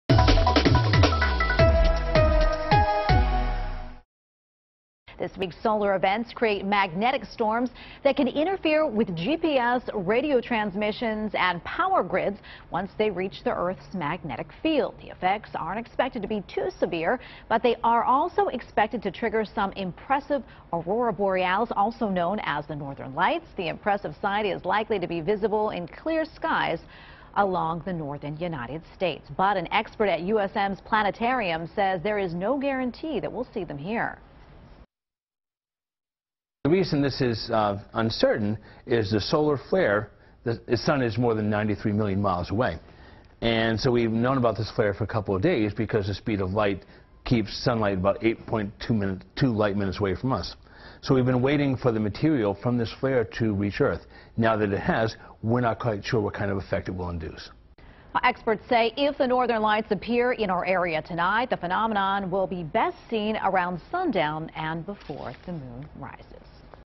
USM astronomer talks about Northern Lights
Two big solar storms aimed at Earth could give Mainers a reason to look to the sky Friday night. But an astronomer from the University of Southern Maine's planetarium said there is no guarantee Mainers will see the Northern Lights.